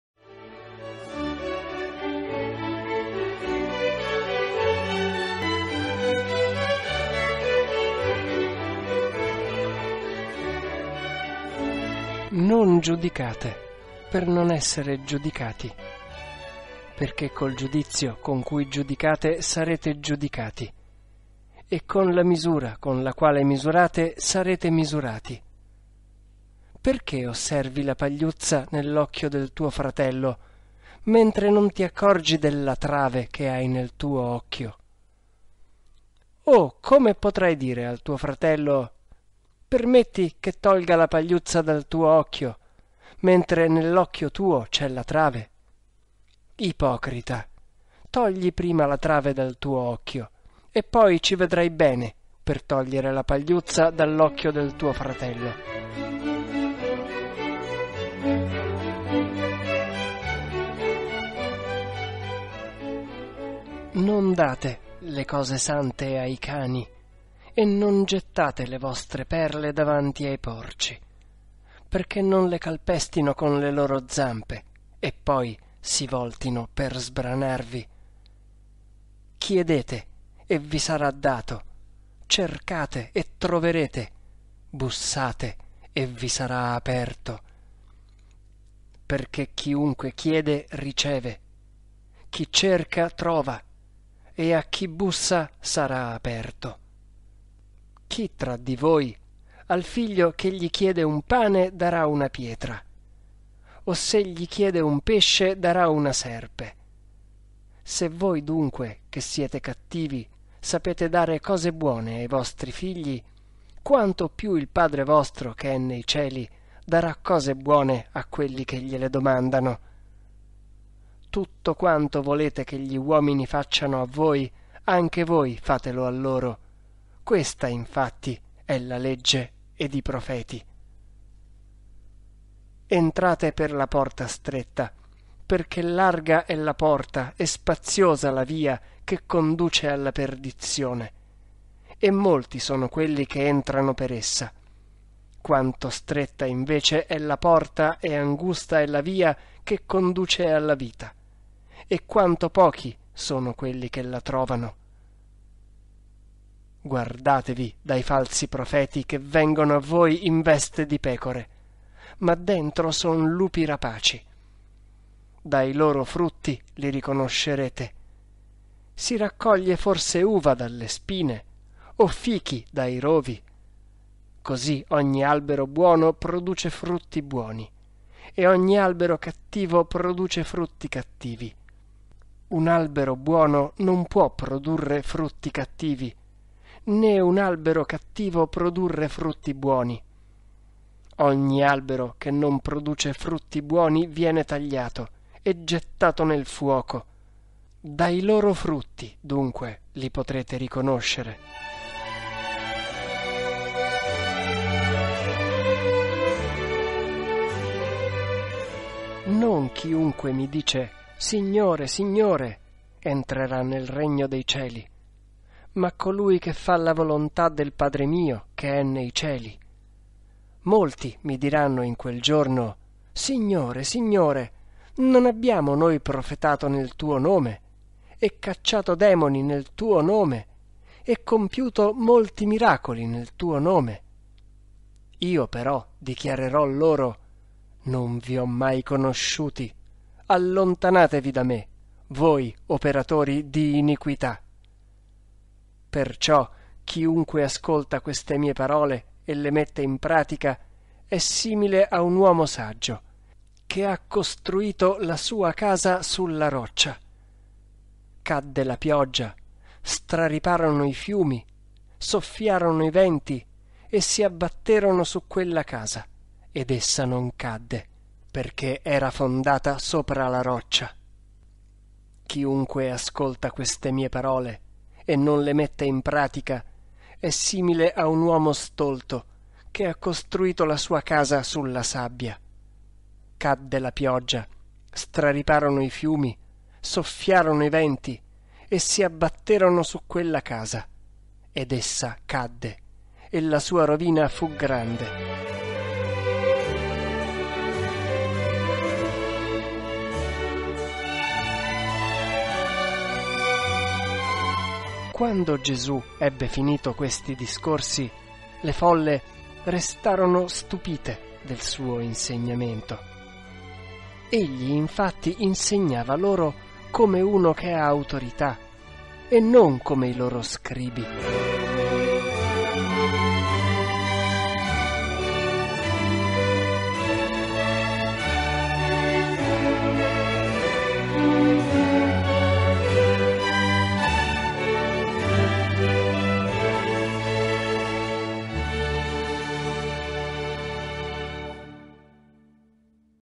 Frasi Vangelo secondo Matteo mp4
Frasi di Gesù tratte dal Vangelo di Matteo per meditare.